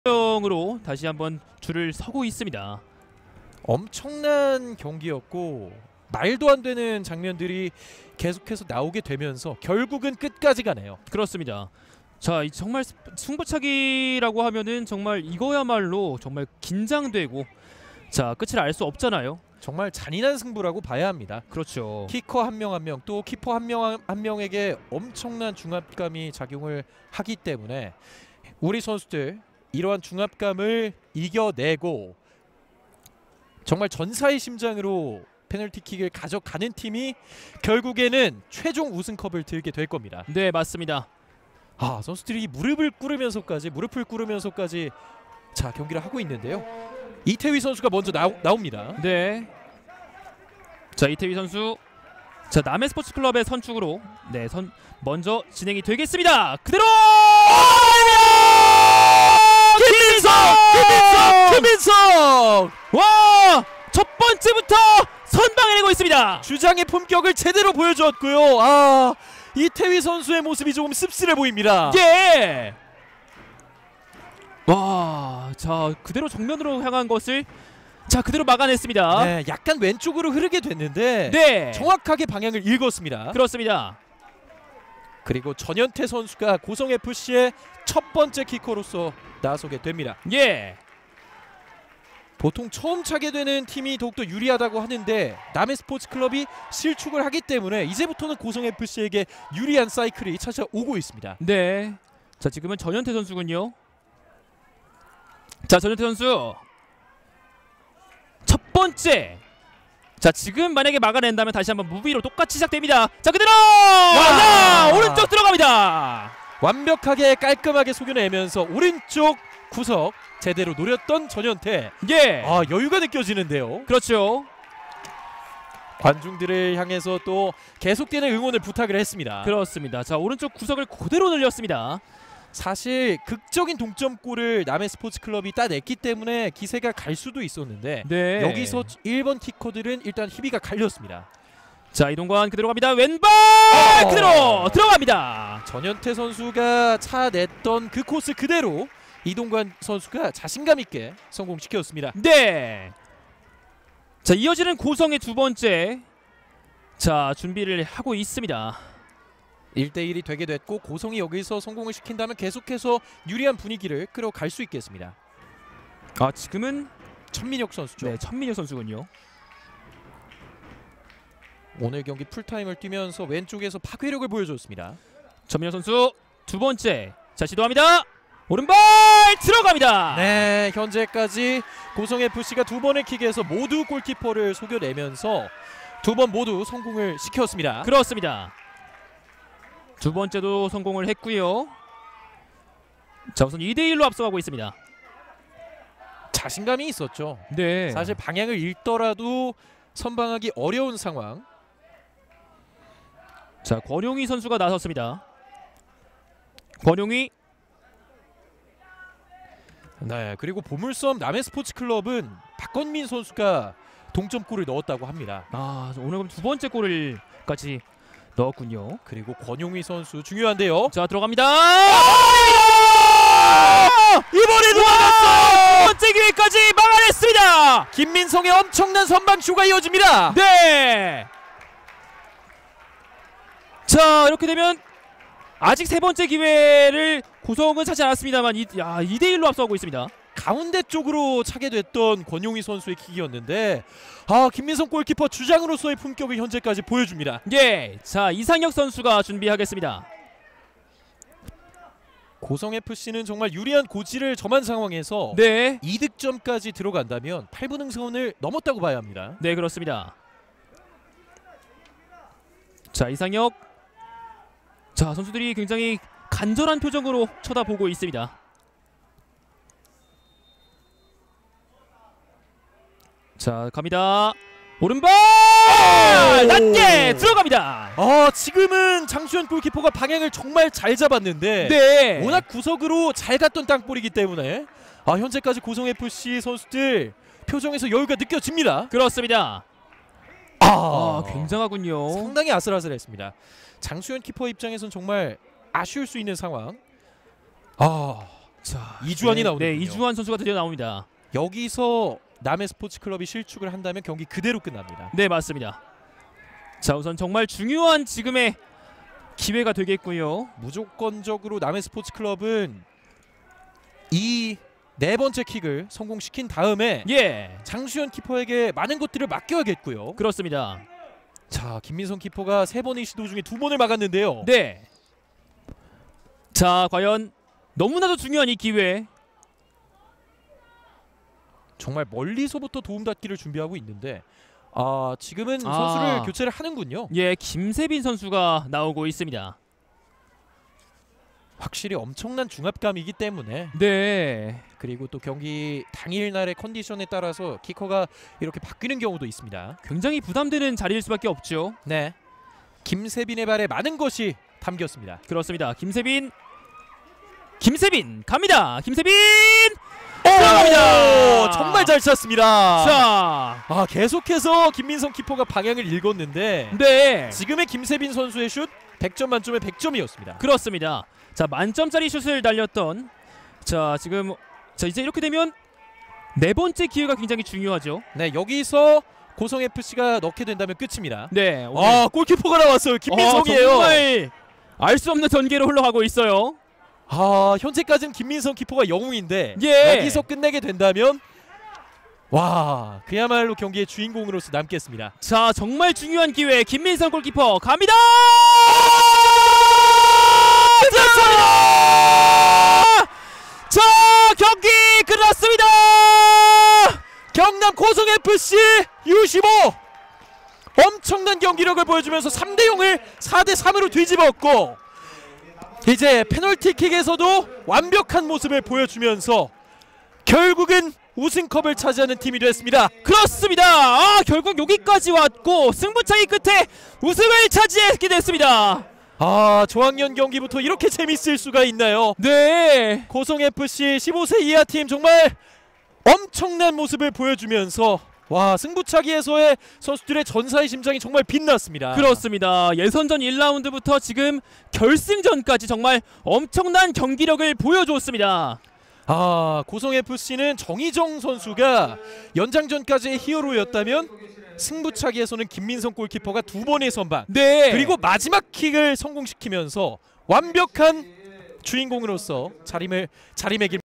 승부차기 풀버전 [저학년] 결승 ㅣ 경남 보물섬남해스포츠클럽 U-15 vs 경남 고성FC U-15 ㅣ 2022 청룡기 전국 중등축구대회 - 22.08.17
승부차기 풀버전  [저학년] 2022 청룡기 결승 ㅣ 경남 보물섬남해스포츠클럽 U-15 vs 경남 고성FC U-15 ㅣ 고성군종합운동장 ㅣ 2022 청룡기 전국 중등축구대회 - 22.08.17